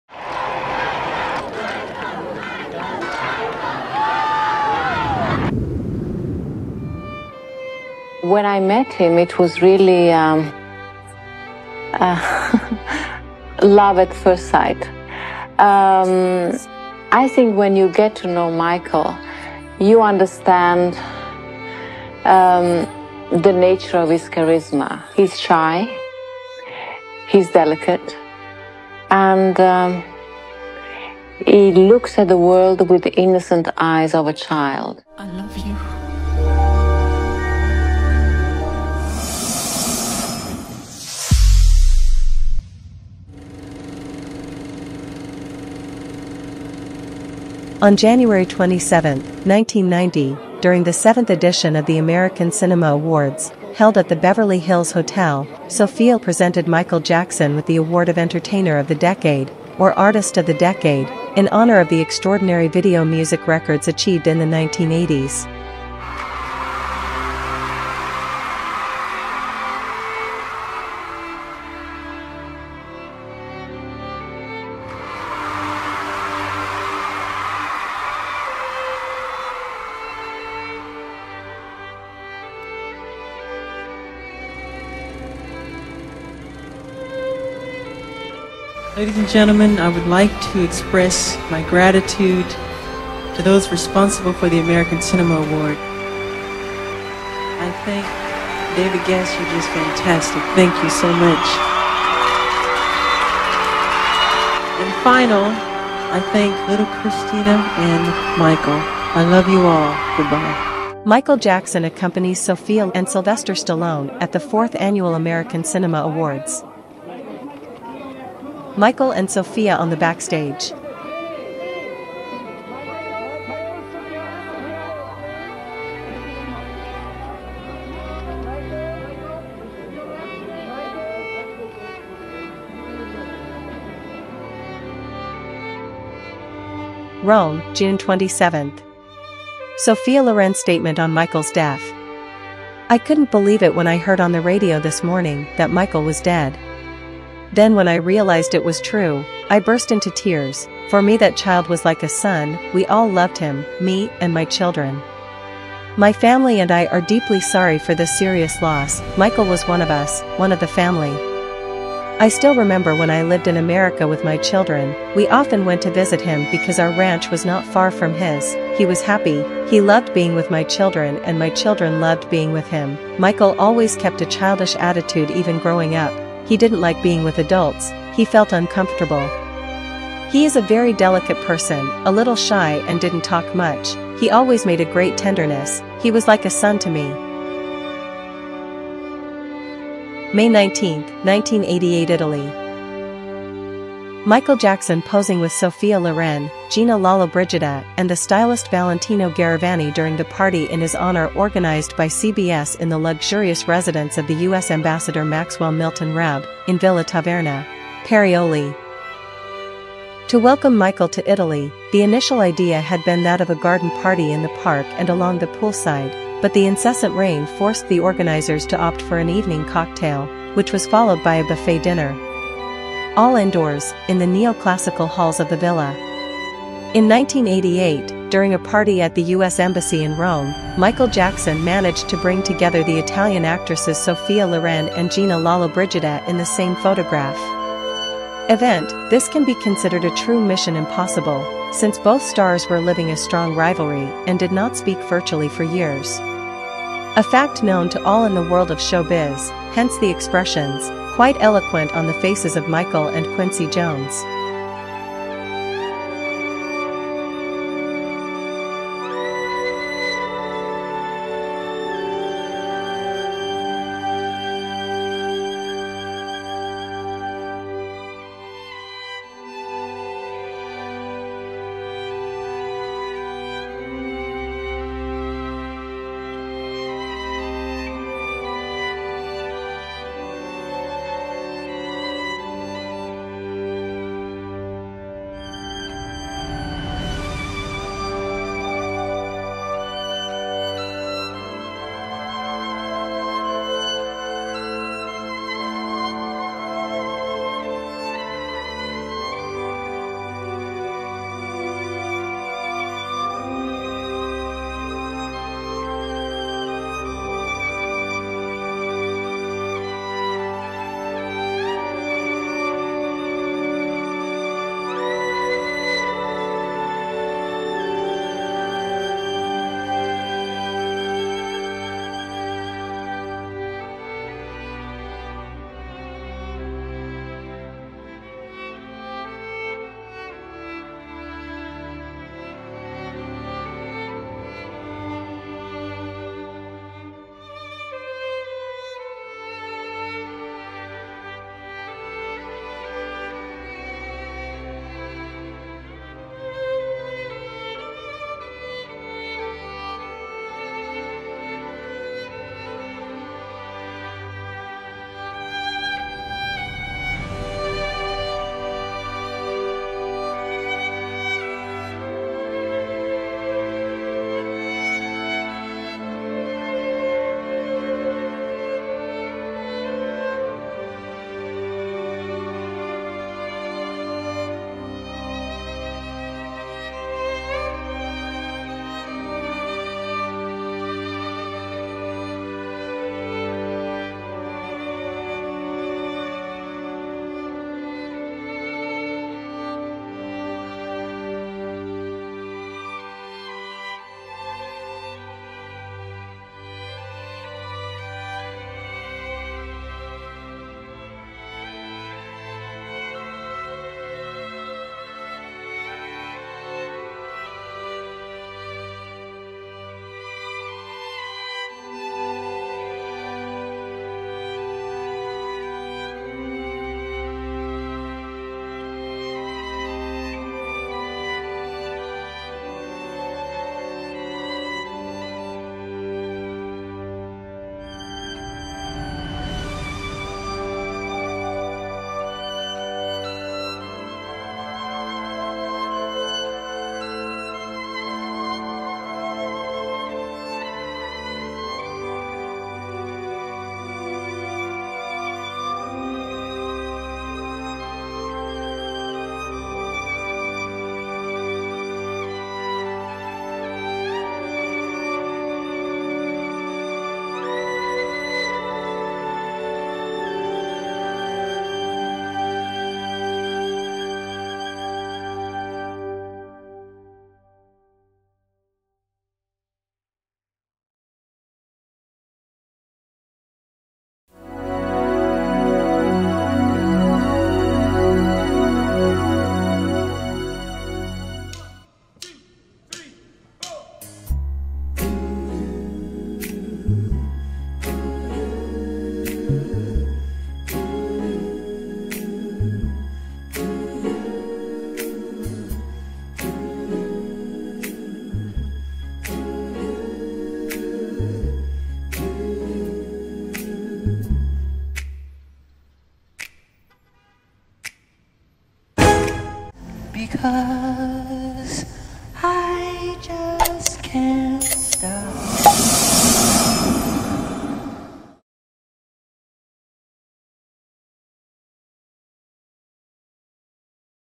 Sophia Loren and Michael Jackson Friendship ♥ღ MJ at the American Cinema Awards 1990 ♥ღ#xyanaღILMOMJ
Sophia Loren was a Michael Jackson's friend.
(Rome, June 27) '' I couldn't believe it when I heard on the radio this morning that Michael was dead.  Then when I realized it was true, I burst into tears: for me that child was like a son, we all loved him, me and my children ''.
"My family and I are deeply sorry for this serious loss, Michael was one of us, one of the family.
I still remember when I lived in America with my children, we often went to visit him because our ranch was not far from his, he was happy, he loved being with my children and my children loved being with him, Michael always kept a childish attitude even growing up, he didn't like being with adults, he felt uncomfortable.
He is a very delicate person, a little shy and didn't talk much, he always made a great tenderness, he was like a son to me! "
- Sophia Loren  -

Michael went to Italy to starting his european tour in 1988.
To welcome Michael to Italy, the initial idea had been that of a garden party in the park and along the poolside; but the incessant rain forced the organizers to opt for an evening cocktail, which was followed by a buffet dinner. All indoors, in the neoclassical halls of the villa.

In 1988, during a party at the US Embassy in Rome, Michael Jackson managed to bring together the Italian actresses Sophia Loren and Gina Lollobrigida in the same photograph.
Event, this can be considered a true "mission impossible" since both stars were living a strong rivalry and did not speak virtually for years.
A fact known to all in the world of show biz, hence the expressions, quite eloquent
on the faces of Michael and Quincy Jones. (Michael Jackson FanSquare • ITALY.)

►1990 Michael Jackson American Cinema Awards

On January 27, 1990, during the seventh edition of the American Cinema Awards, held at the Beverly Hills Hotel, Sophia Loren presented Michael Jackson with the award of Entertainer of the Decade - or Artist of the Decade - in honor of the extraordinary video music records achieved in the 1980s.

Looking for the perfect MJ Christmas Gift for someone special?
This MJ Merchandise is the perfect one!
For true Applehead lovers.

🔴 Etsy Shop
MagicStrass - Instant Digital Templates and Rhinestone Designs